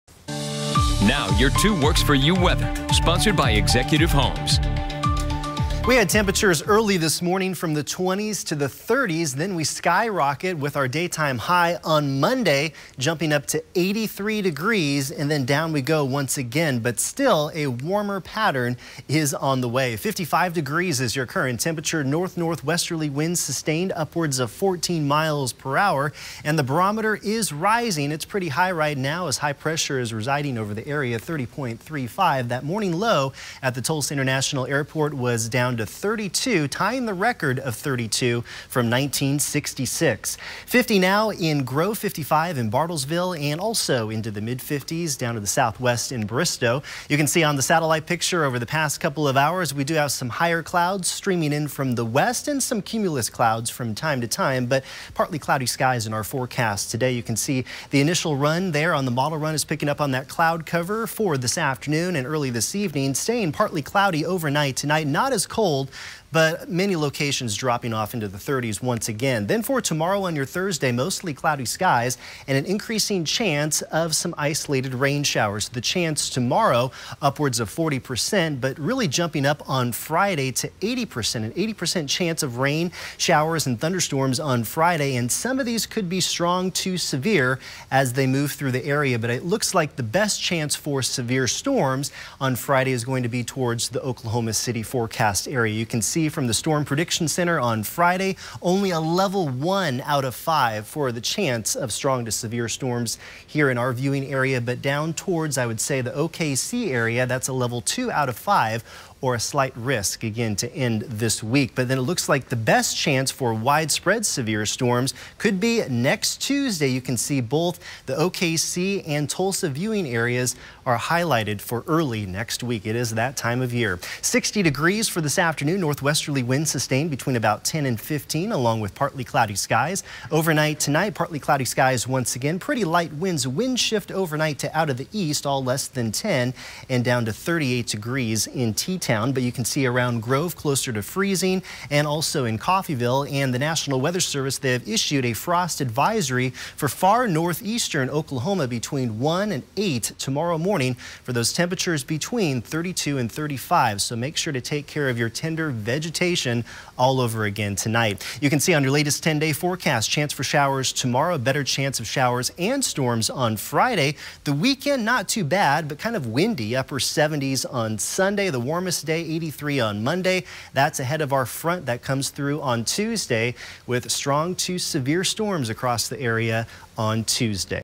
Wednesday Noon Weathercast
Brandon has a look at the changes ahead.